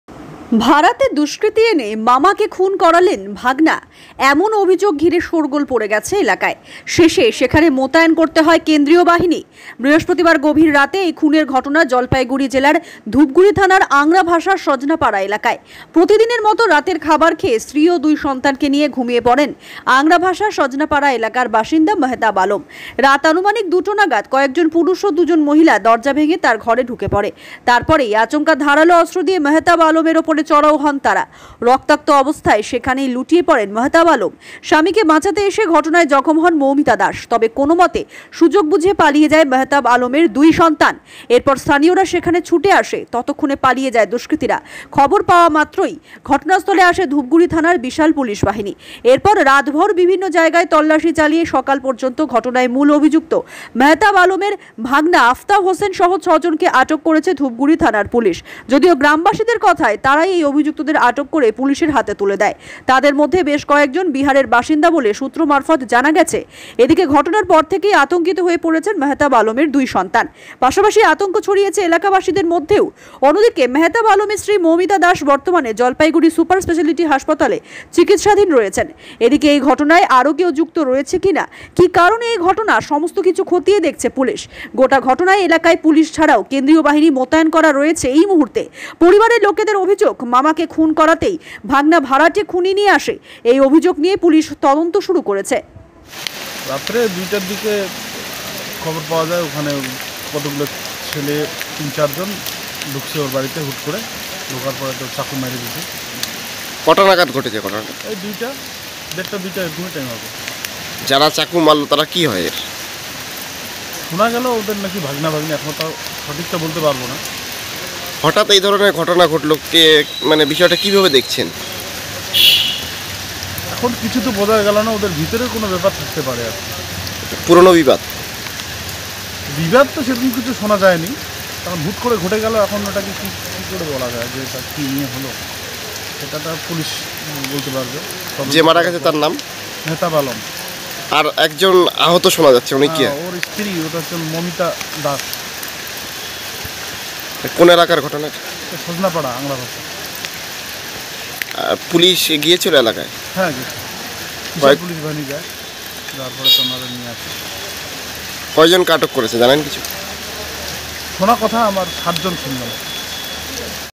বিহার থেকে দুষ্কৃতী এনে মামাকে খুন!
uncle was killed by criminal #6 people arrested including nephew Killed in a land dispute